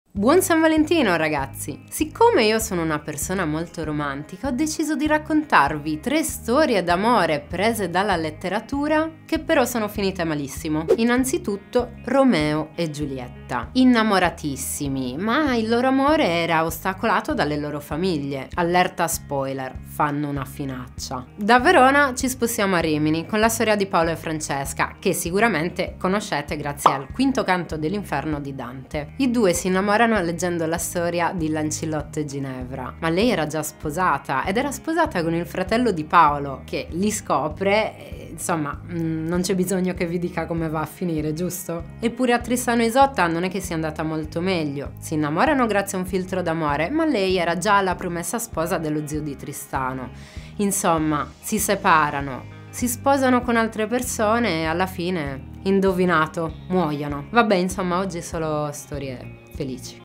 3 storie d'amore finite male
San Valentino, 3 bellissime storie d'amore prese dalla letteratura... che però sono finite malissimo